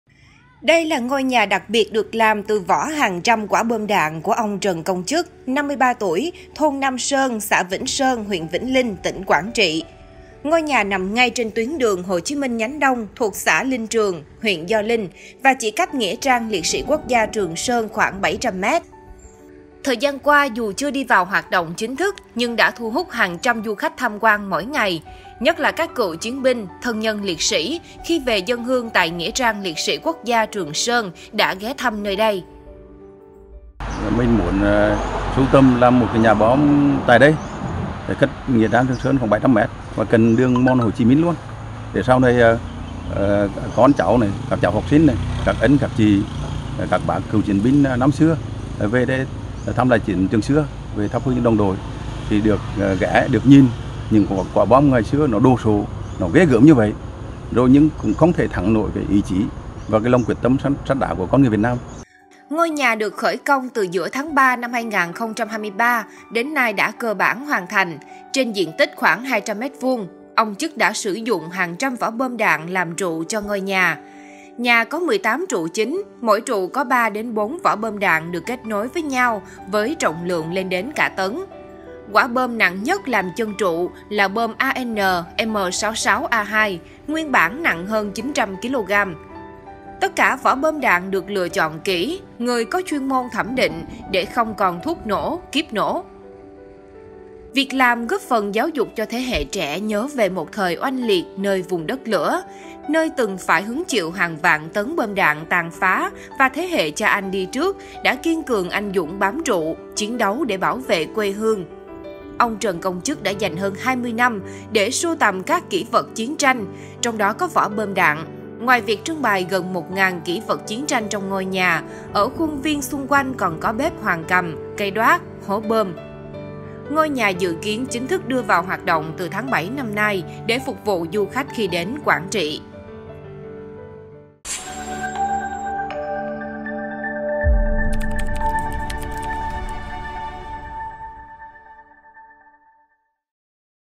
Ngôi nhà đặc biệt được dựng lên từ hàng trăm vỏ bom đạn
Ngôi nhà nằm ngay trên tuyến đường Hồ Chí Minh nhánh Đông thuộc xã Linh Trường, huyện Gio Linh và chỉ cách Nghĩa trang liệt sĩ quốc gia Trường Sơn khoảng 700m.